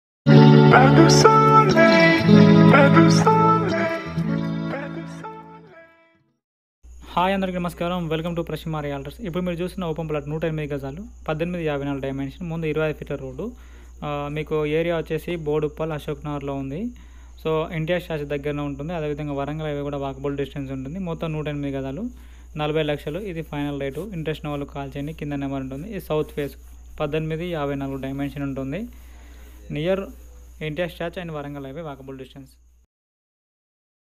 Property for sale in Hyderabad boduppal | Reasonable rate | 108 Sq Yards | Prashi Maa Realtors
This Property Total 108 Sq Yards Dimension 18 & 54 Front road 25 feet road
Rate 40Lakhs final rate near NTR statue Ashok nagar warangal highway walkable

SOLD OUT

- 108 sq yards south face

- 18 & 54 Size

- ashok nagar

-40lakh rate Final Rate

- Boduppal Hyderabad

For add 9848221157